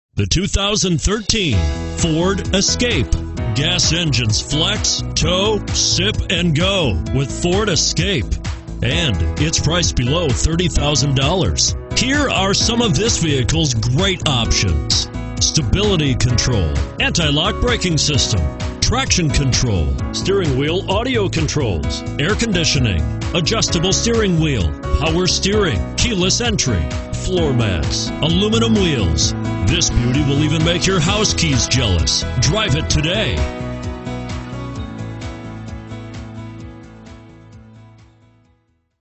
2013 FORD ESCAPE Taylor, MI 76999
2013 FORD ESCAPE Taylor, MI
Stock# 76999

Taylor Ford
10725 South Telegraph

Don't miss this 2013 Ford Escape. It's equipped with Automatic transmission and features a Tuxedo Black exterior. With 1 miles, you'll want to take this car home. Make a great choice today.